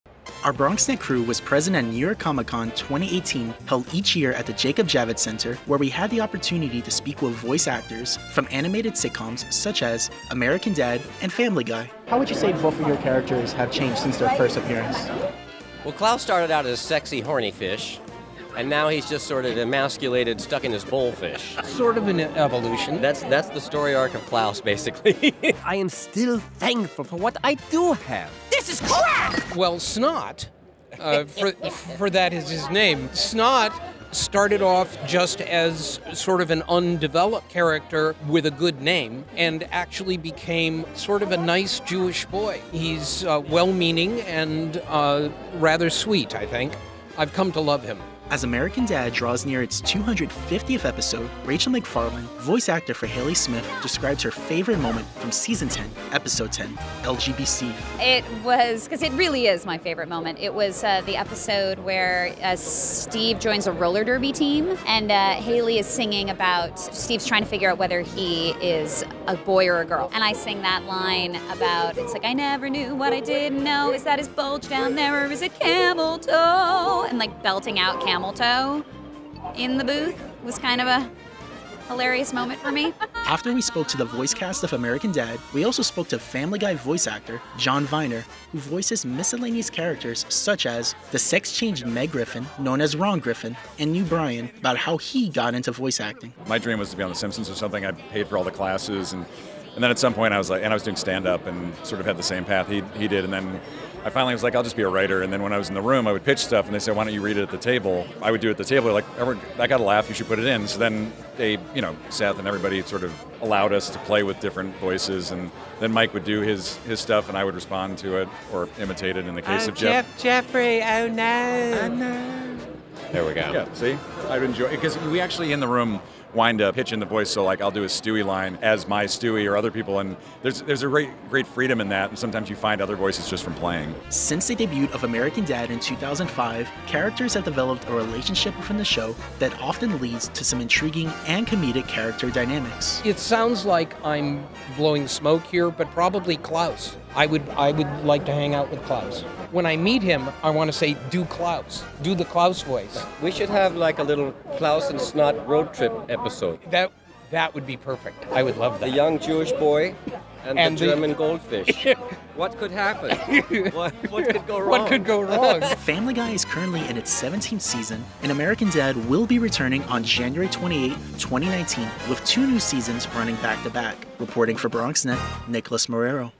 NYCC 2018 American Dad & Family Guy Voice Cast Coverage
The voice cast of American Dad & Family Guy shares their thoughts on being a voice actor and the relationship they have with their characters.